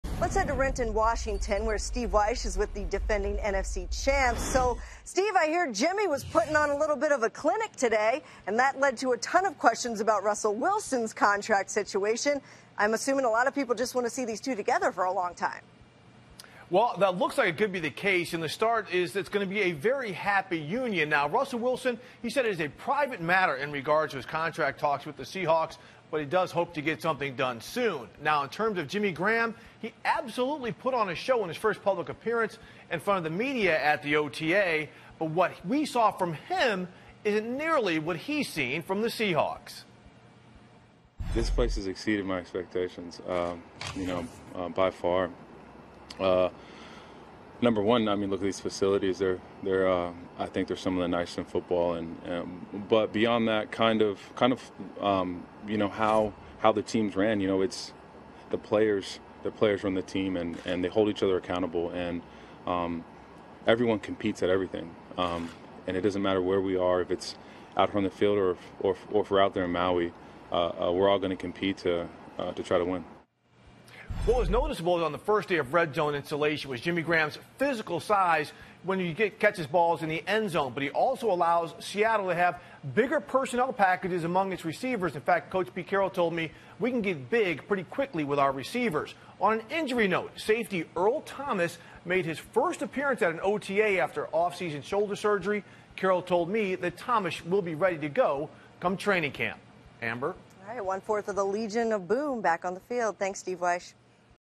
Jimmy Graham putting on a show at Seahawks OTAs
Seattle Seahawks tight end Jimmy Graham putting on a show at Seahawks OTAs